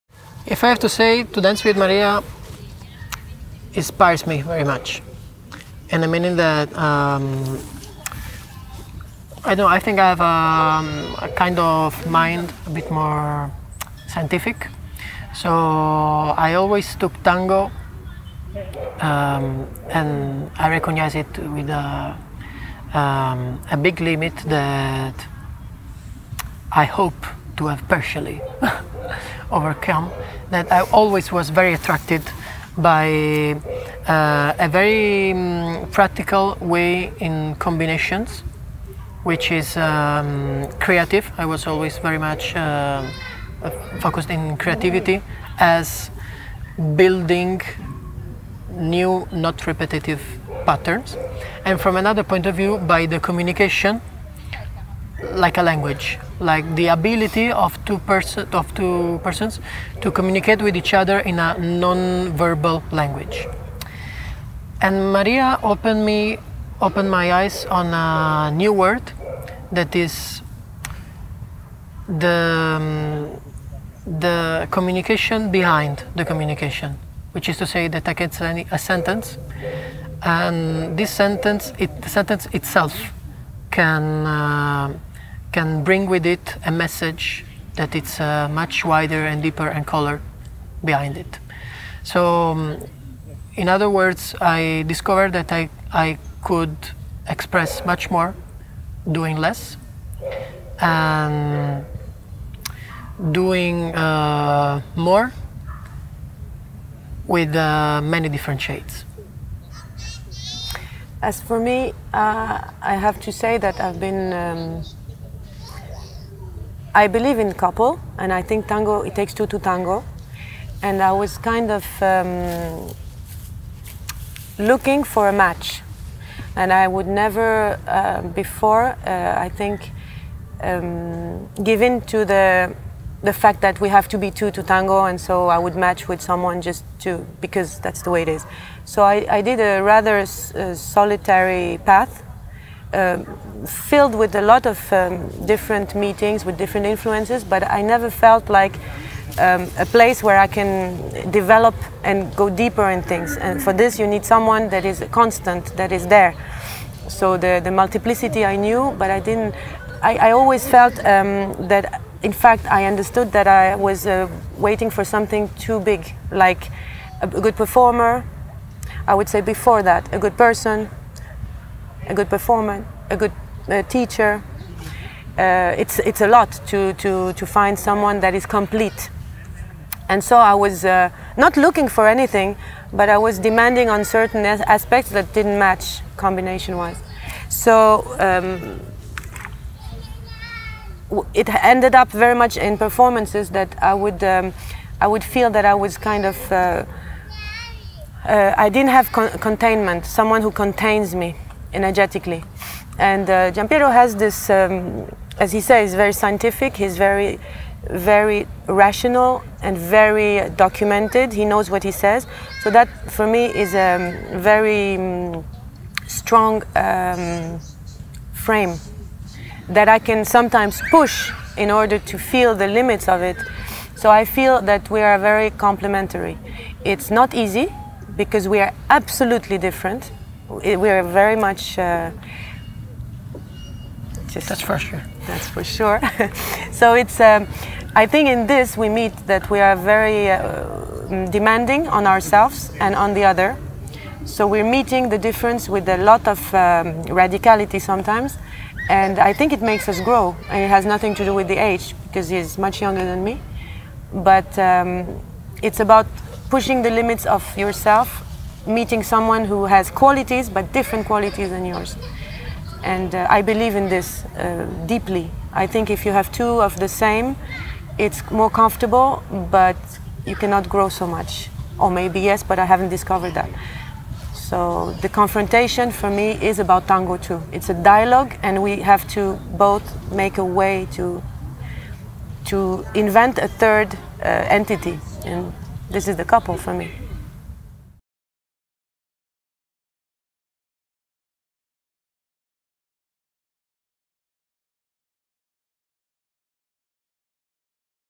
Maria and Gianpiero about dancing with each other » 030tango Short
In this 030tango Short Maria Filali and Gianpiero Galdi tell us what makes dancing with each other special!

Recorded on 2016/08/20 at Tango Cazino Festival 2016